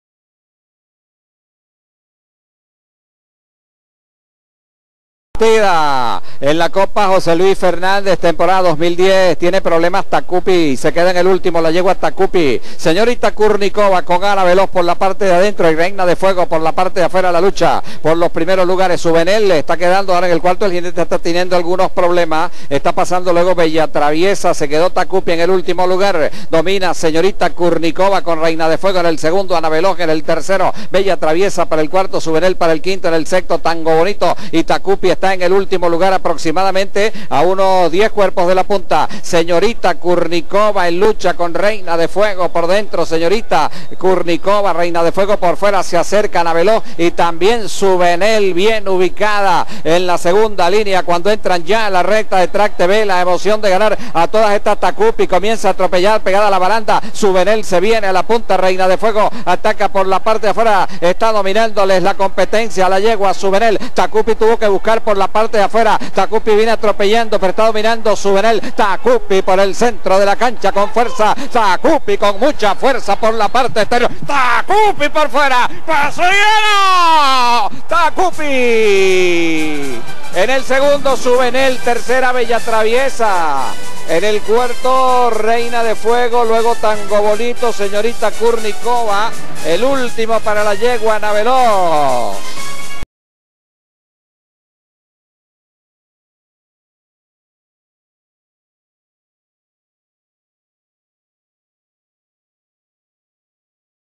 Copa José Luis Fernández 2010 - La Rinconada
Tremenda demostración de Takupi, hija de Slaytheodds de La Quebrada, que a pesar de los inconvenientes en la partida pudo recuperarse para atropellar con mucha fuerza en la recta y dominar al final, a las órdenes de Emisael Jaramillo y el entrenamiento de Julio Ayala. Stud "Star Light".